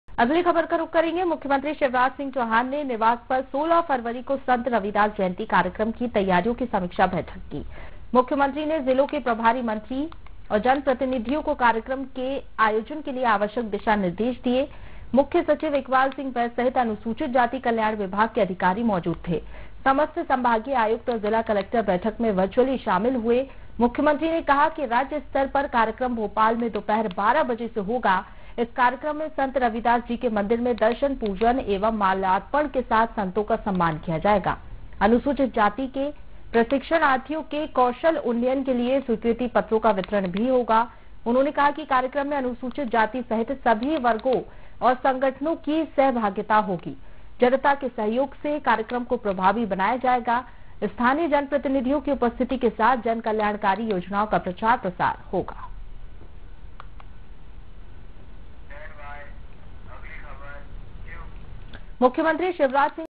भोपाल : सीएम शिवराज सिंह ने की संत रविदास जयंती कार्यक्रम की समीक्षा-News World
भोपाल : सीएम शिवराज सिंह ने की संत रविदास जयंती कार्यक्रम की समीक्षा